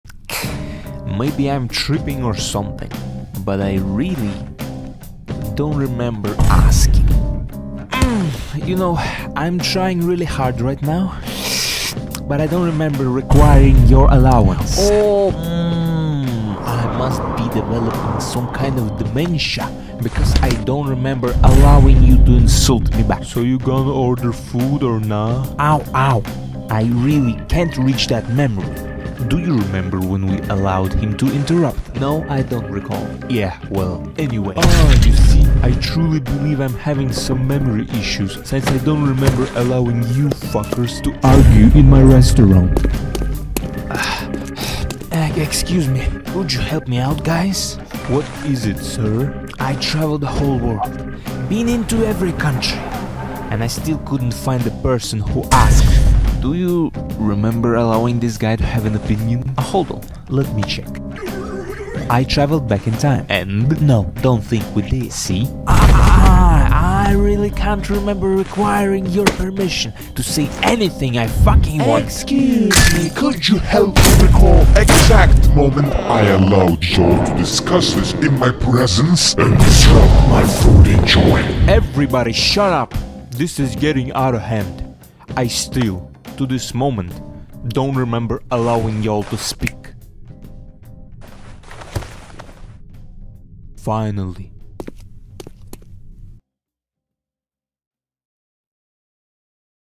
SMG12 SALLY and combine soldier peter argue in a resturaunt
read description

audio from & so you know what poses the characters in our rp chat were supposed to be in

i will do a alternate version of this using gmod with some other characters